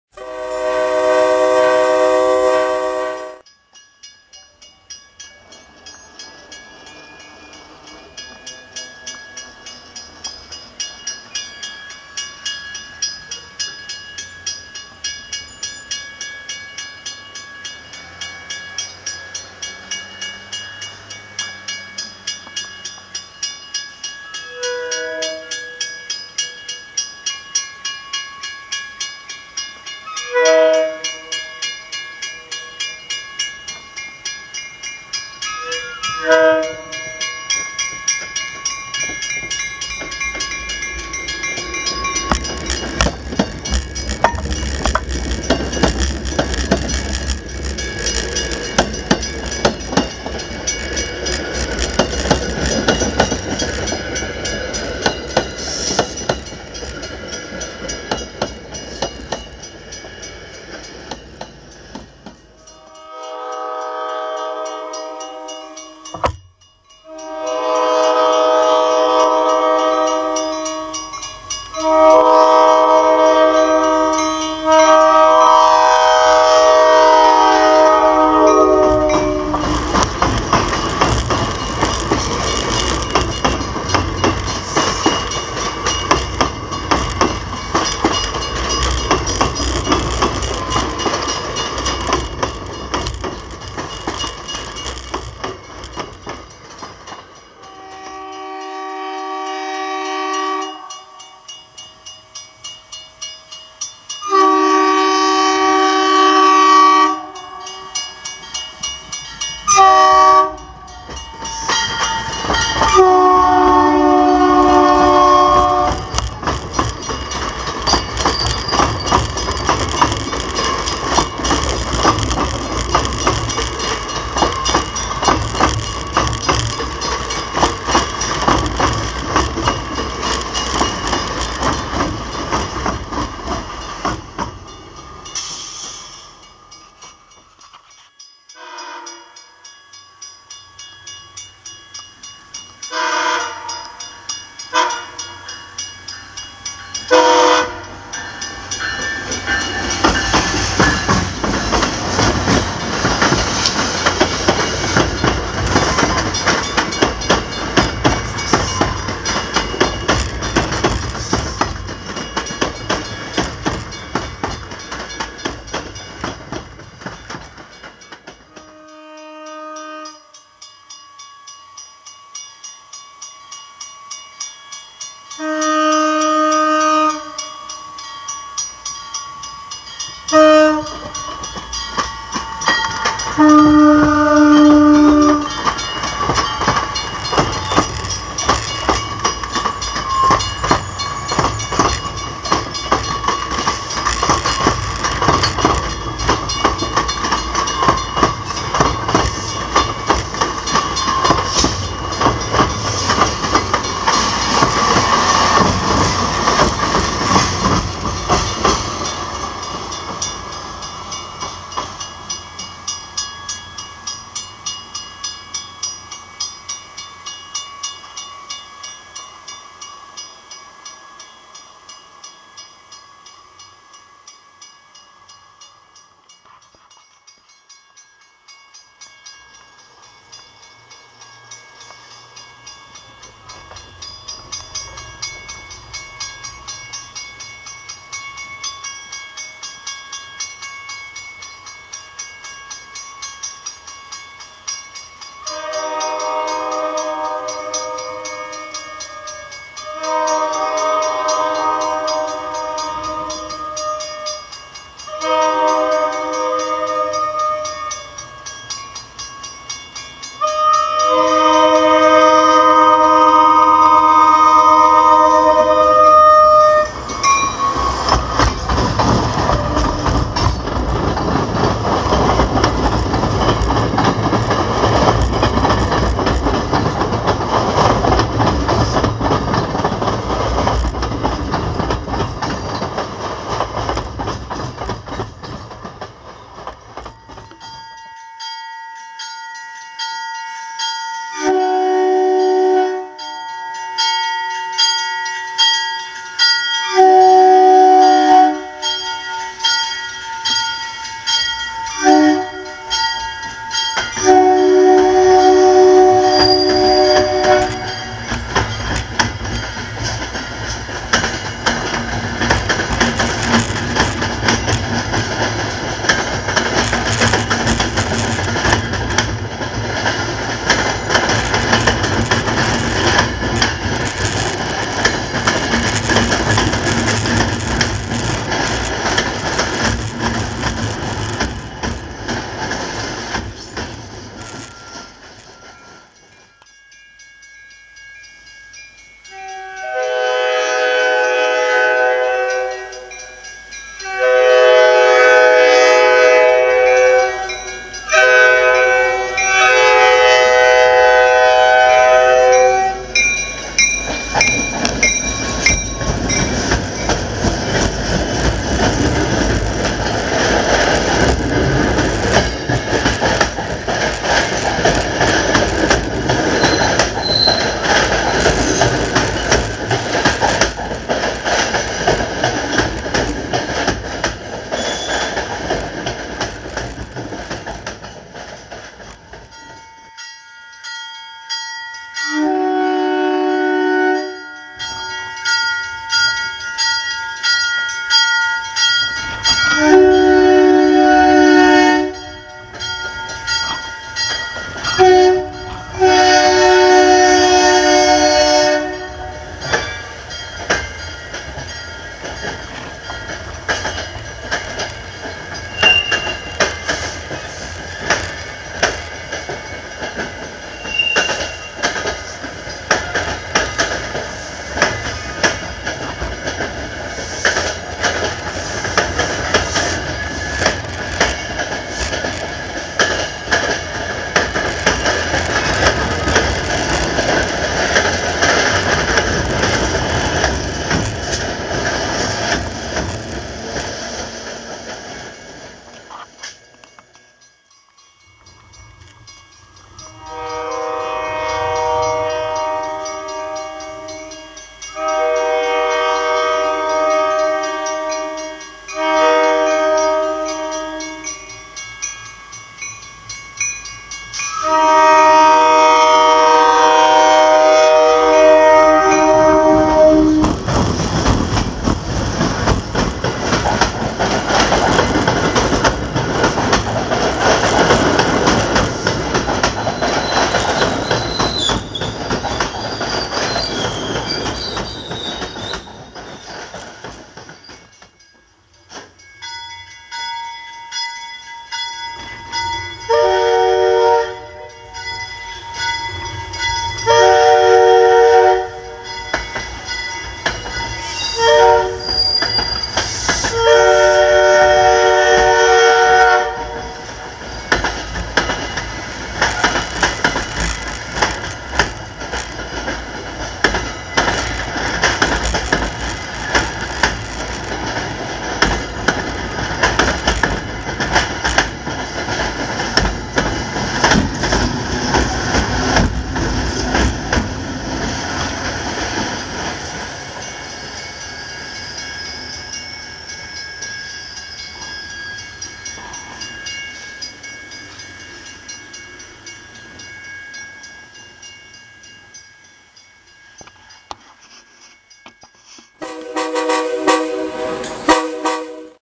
Morning Rosemary St.-Afternoon Heights Railfanning in Needham
Just in case the title doesn't make sense it the Morning Railfanning at Rosemary St. Crossing and Afternoon Railfanning at Needham Heights Station. That's what the title means so please don't ask any questions that's related to title misreadings. Finally after at lease 2 Months, 011 is finally back on the South Station MBCR lines, but i'm still not sure if they're running 010 or not. Anybody who knows the answer to the 010 question, please post a comment about it, otherwise, don't worry about it. Lots of good horns except 1509, sadly with 011 b/c it ruins the fun, but the engineers are all really nice when they saw me. If you have any questions, not related the the topic that I said earlier in the description, feel free to post a comment about it and it will get answered accordingly.